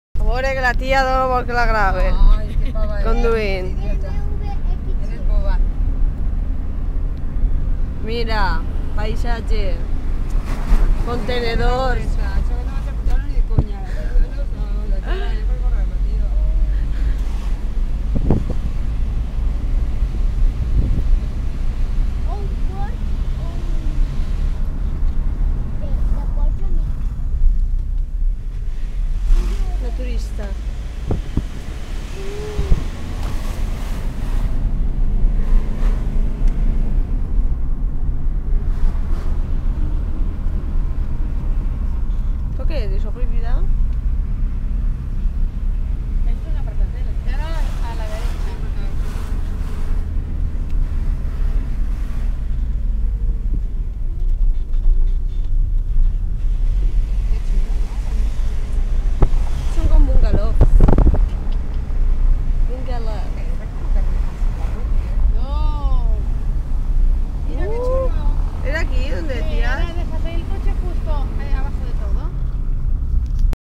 Perdidas por ciutadella de menorca
Sara y yo nos perdimos por ciutadella , fue una aventura fantastica que nos dejo tiempo para disfrutar mas aún de lo fantastaica que es menorca. Al final de trayecto encontremos lo mejor ¡¡¡ adivina que es?